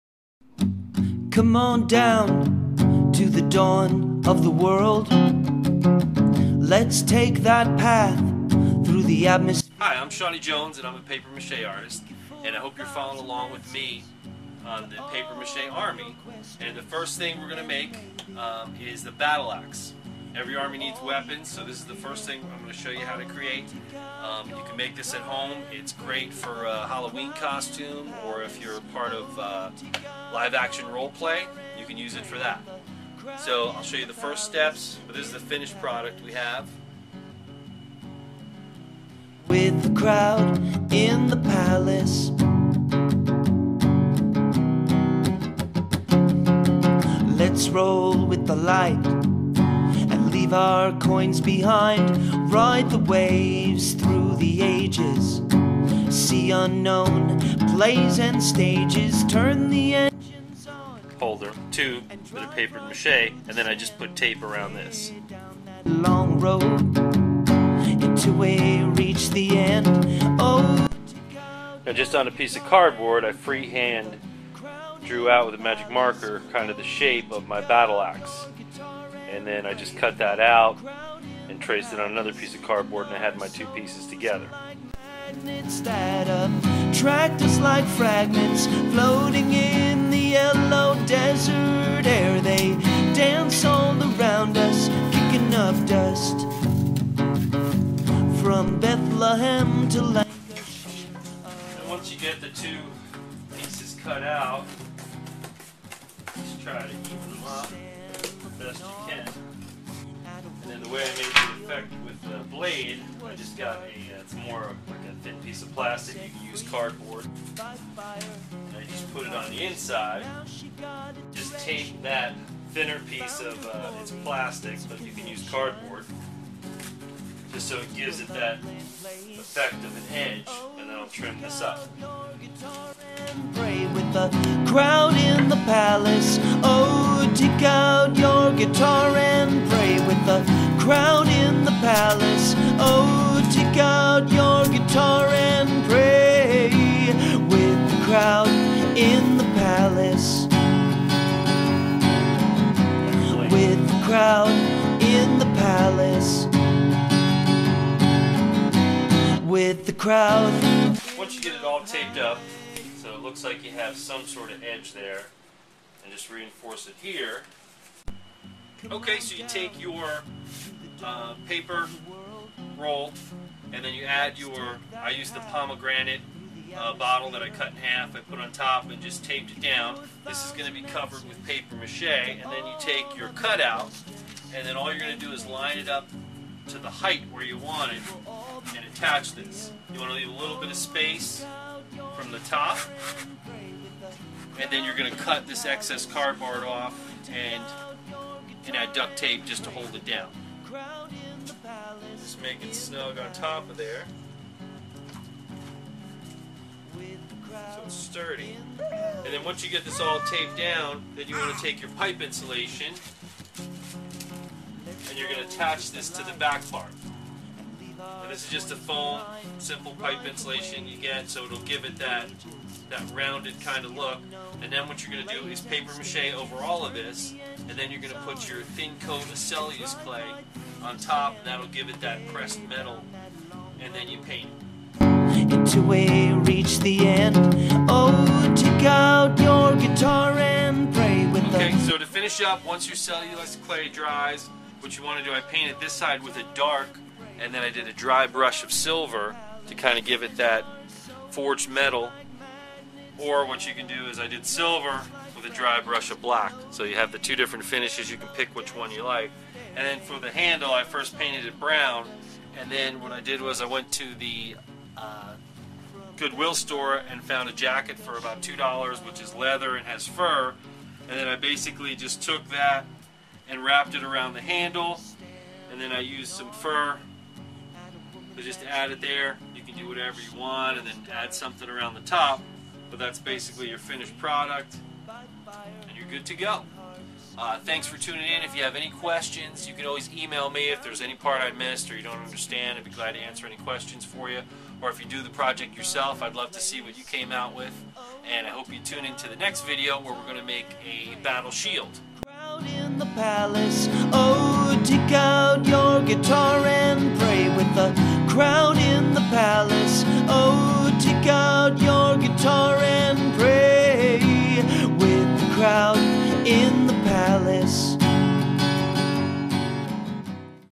Paper Mache Battle Axe
Every Army needs weapons, learn step by step how to make a heavy battle axe, look for my paper mache book